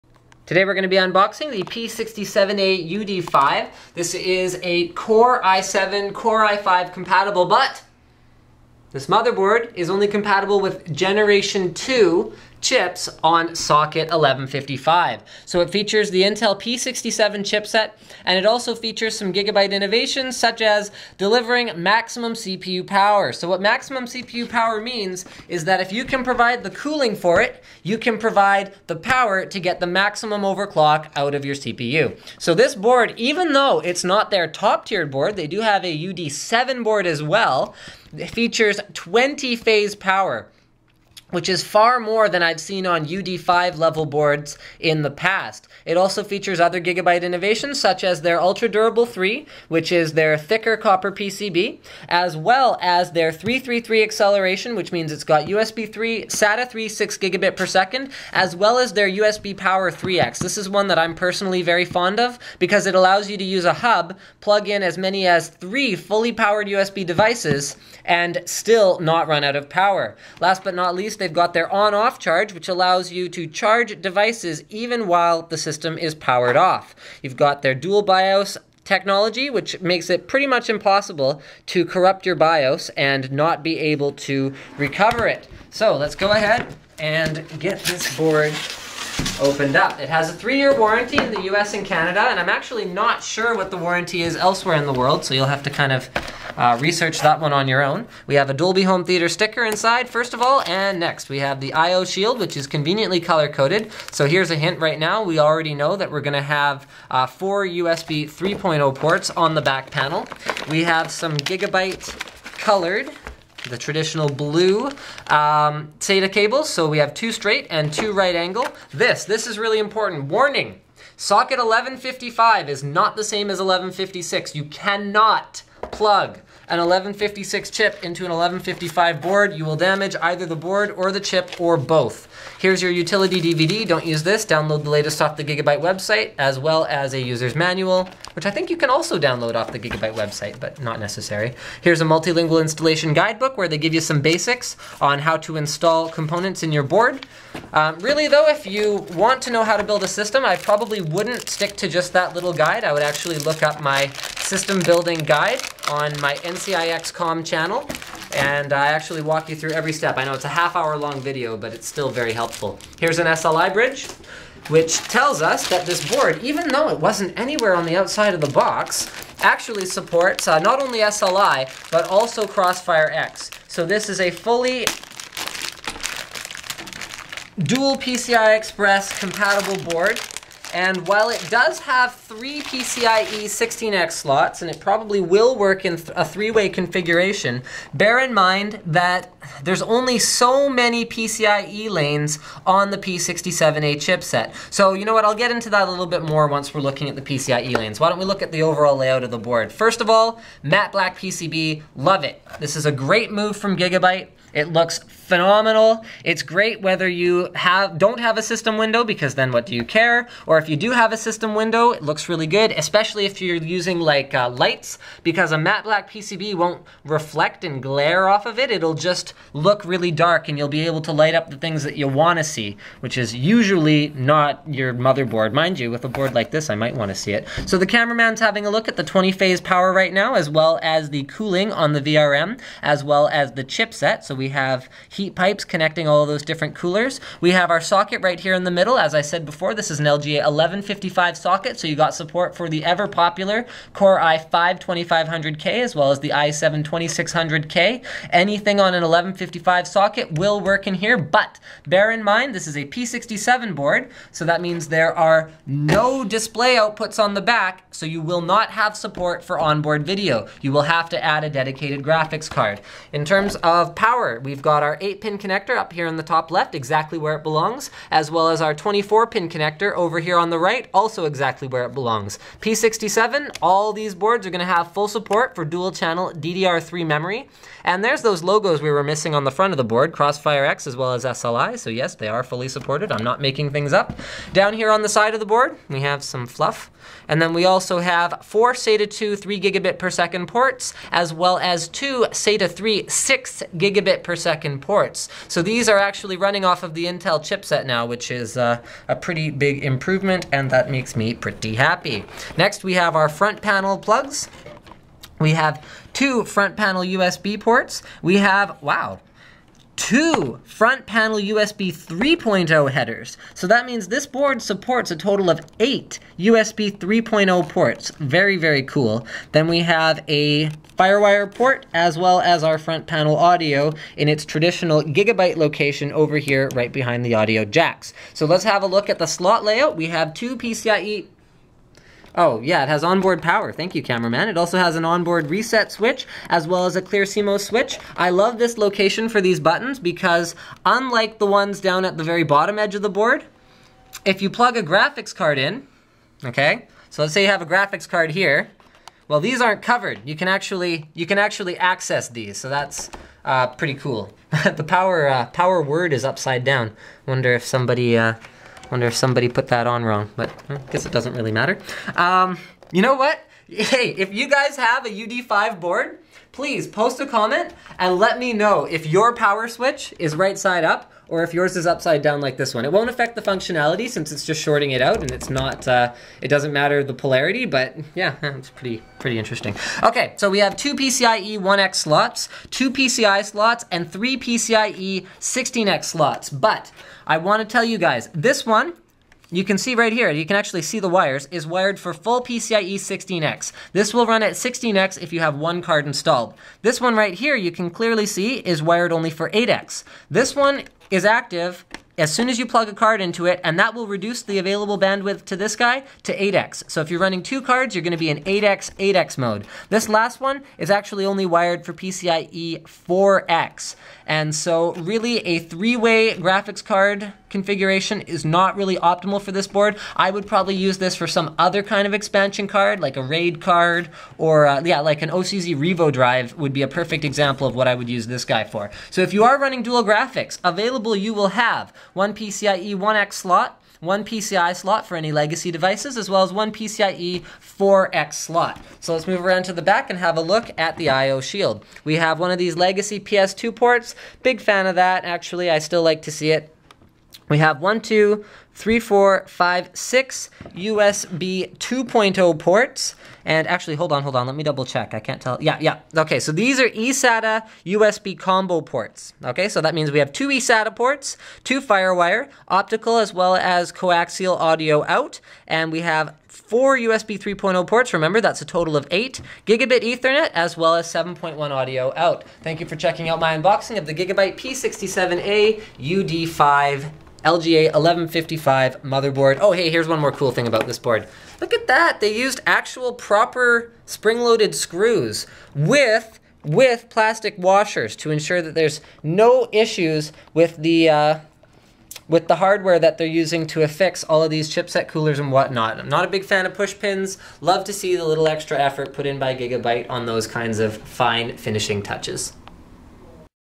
Gigabyte P67A-UD5 P67 LGA1155 Sandy Bridge SLI Motherboard Unboxing & First Look Linus Tech Tips
This is positioned directly below the P67A-UD5. It has slightly fewer features, but also a lower price.